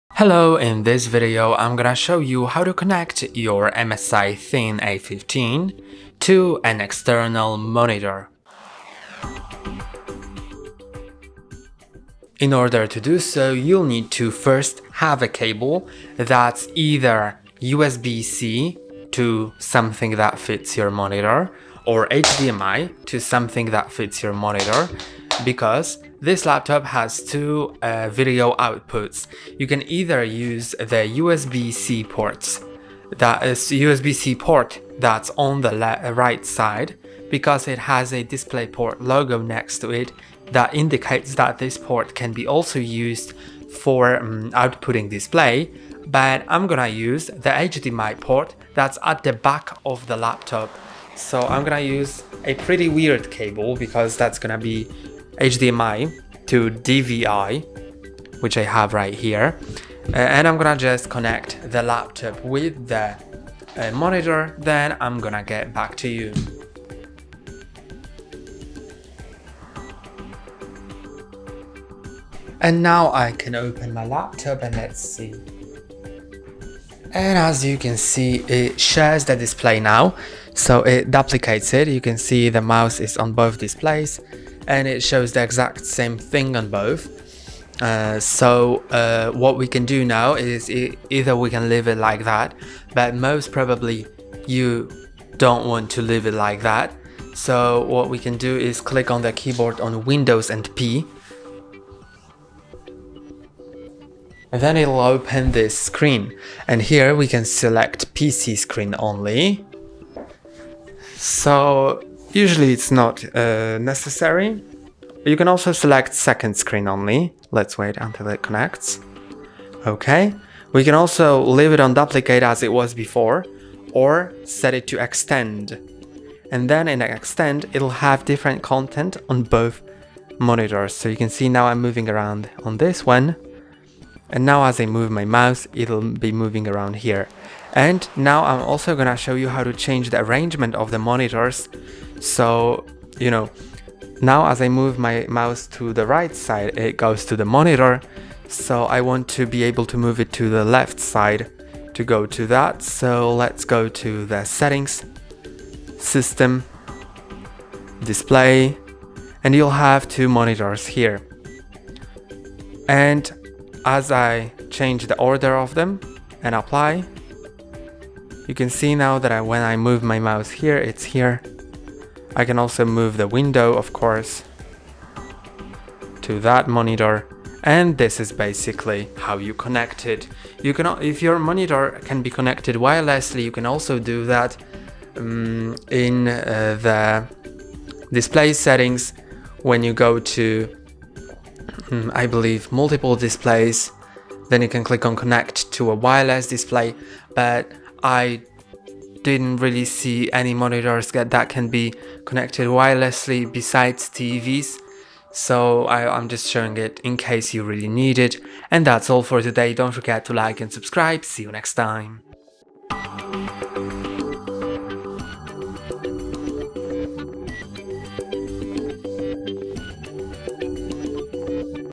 How to Connect an External Display to MSI Thin A15 - Connect a Monitor
Looking to connect an external display to your MSI Thin A15 laptop? This video walks you through the process step by step, covering HDMI and other available ports, display settings, and troubleshooting tips. Learn how to extend or duplicate your screen for gaming, productivity, or a multi-monitor setup. Get the most out of your laptop’s display capabilities with ease.

How to connect an external monitor to MSI Thin A15? Why is my MSI Thin A15 not detecting an external display? What are the best settings for dual monitors on MSI Thin A15?